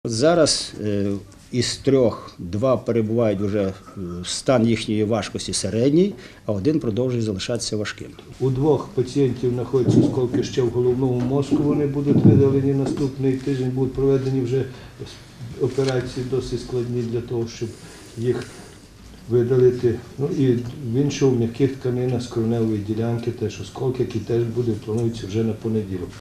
Вибух на полігоні у Рівному: пораненого військового з Хмельниччини прооперували
Військовий з Хмельниччини, який отримав поранення під час вибуху міномета на Рівненському полігоні, після операції перебуває у стані середньої важкості. Про це сьогодні повідомив керівник військово-медичного центру Західного регіону Іван Гайда. Військовий з нашої області та ще 2 поранених на полігоні перебувають у відділені інтенсивної терапії. Наступного тижня їх прооперують ще раз, адже у тілі бійців залишились вогнепальні уламки.  Загрози їхньому життю наразі немає.